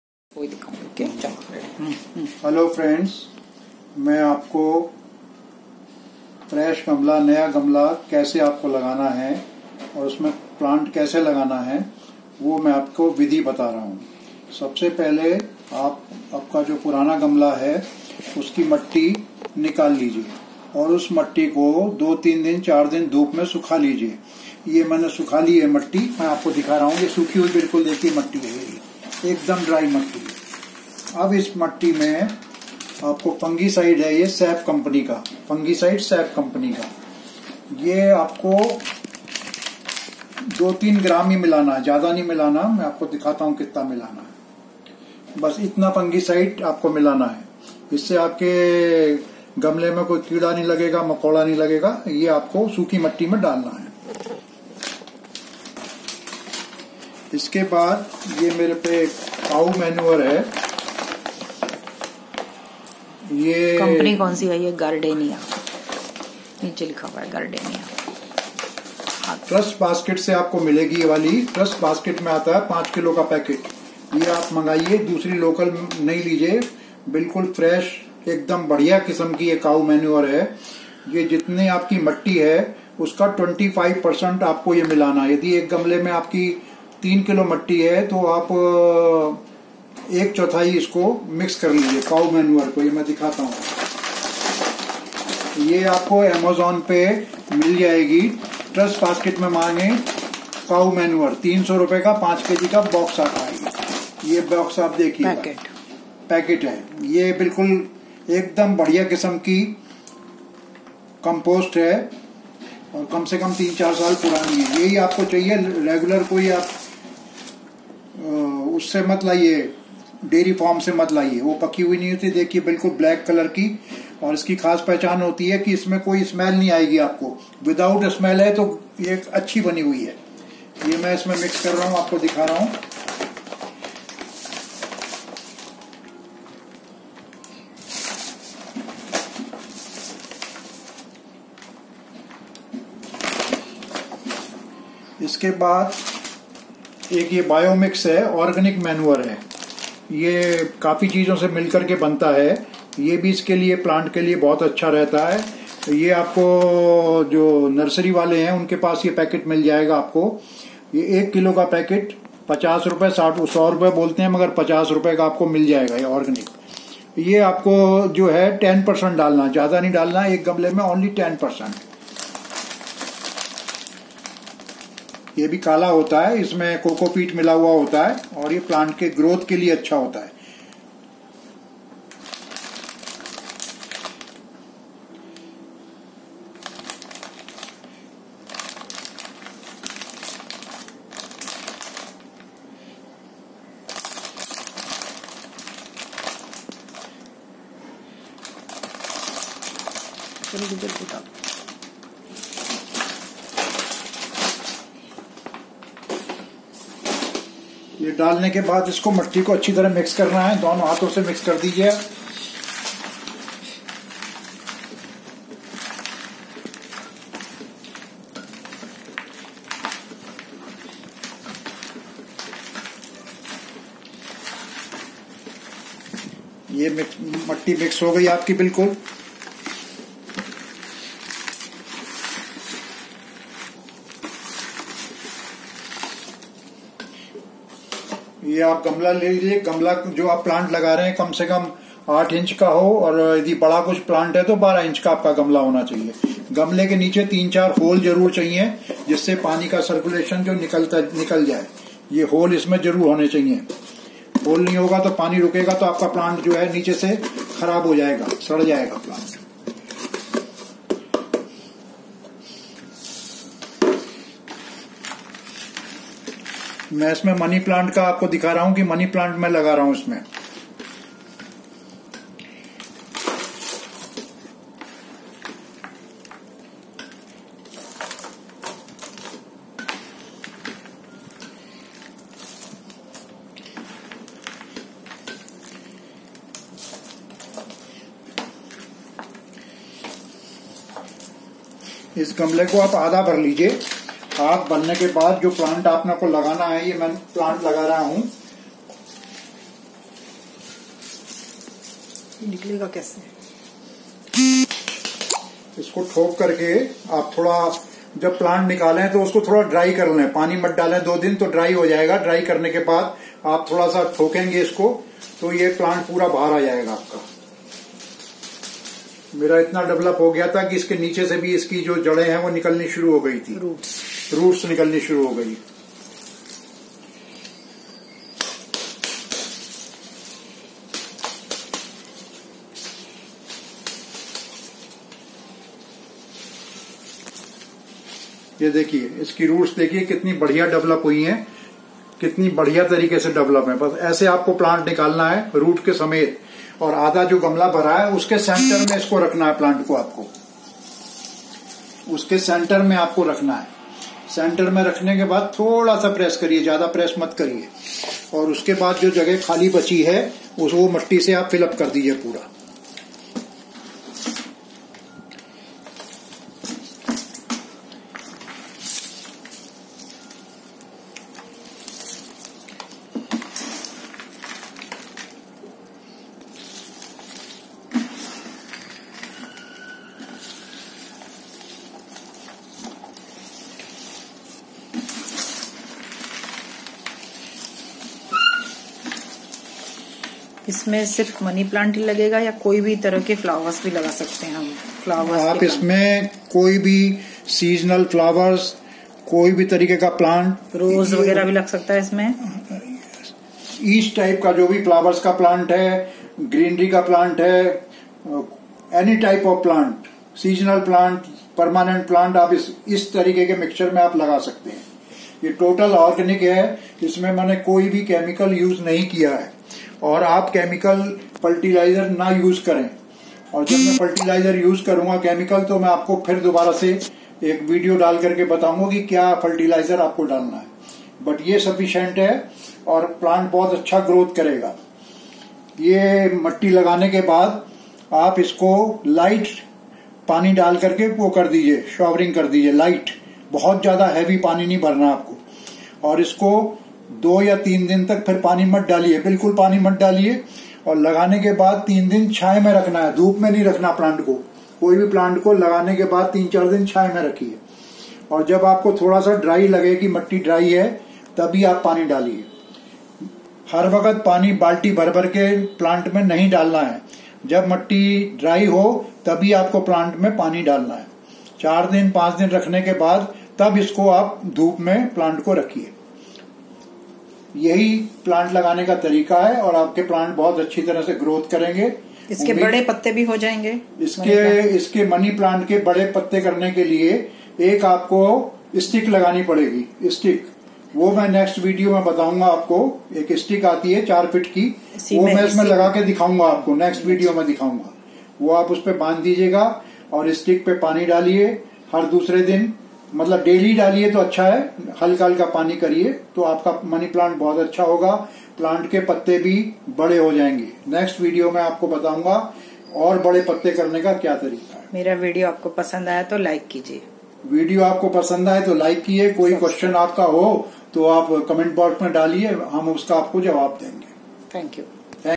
Preparing a Planter
Hello Friends. Here we are showing the process of preparing a planter.
We have used only organic Bio mix, cow dung manure & fungicide with the soil.
Bio Mix is an organic mixture of fertilizer & Cockopit. It helps the roots grow very easily inside the soil. In this type of Planter, you can plant any type of sapling like floral plants, leafy plants or vegetables as well. Cow Manure can be ordered from Trust basket.